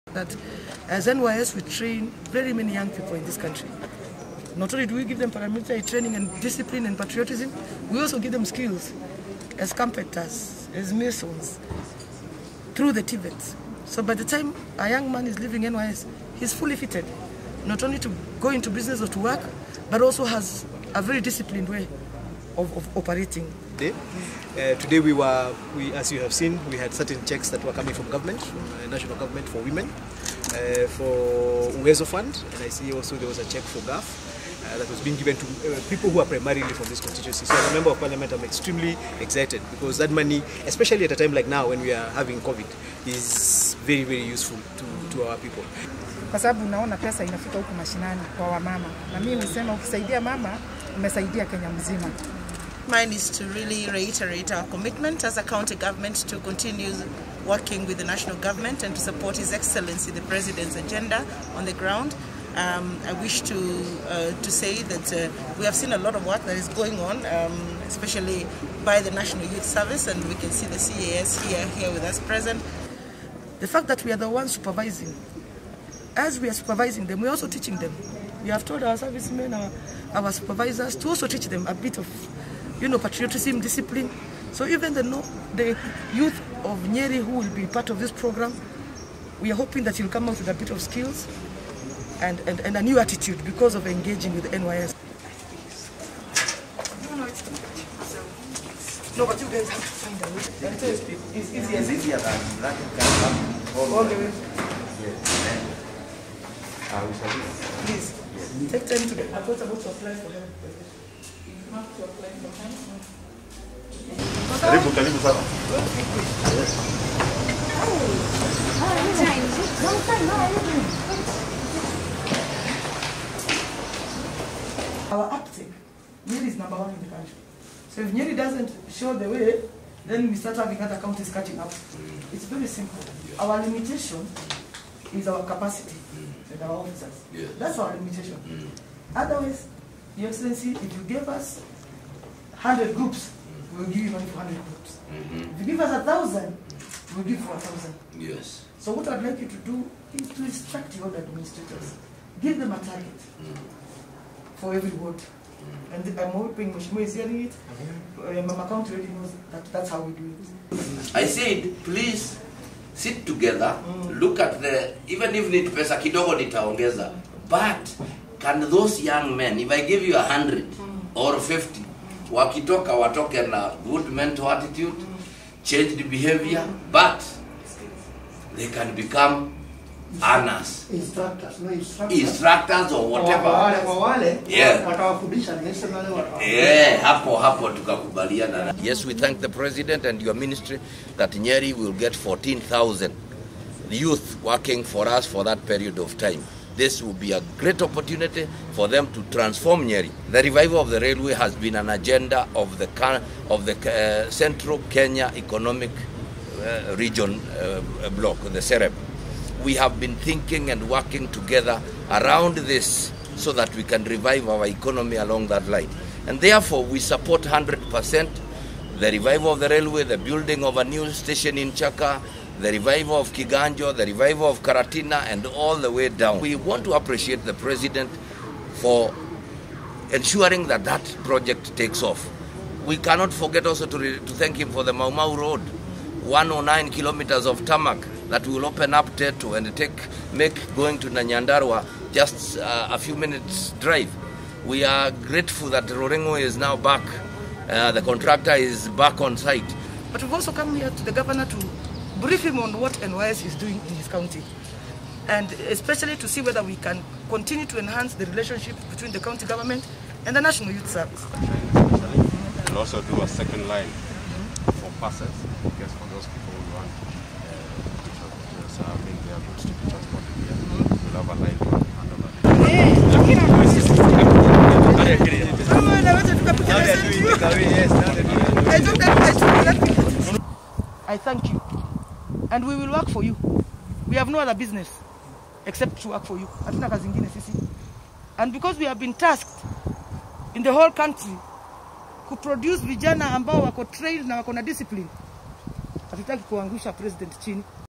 INSPECTION OF MINISTRY PROJECTS AND ISSUANCE OF FUNDS IN NYERI COUNTY
CAS HON. SHEBESH INSPECTION OF THE  REHABILITATION WORKS OF 233KM NAIROBI -NANYUKI RAILWAY LINE BEING UNDERTAKEN BY THE NATIONAL YOUTH SERVICE (NYS) on 12TH JUNE, 2020. DURING HER VISIT SHE ALSO ISSUED AFFIRMATIVE FUND CHEQUES WORTH OVER 63 MILLION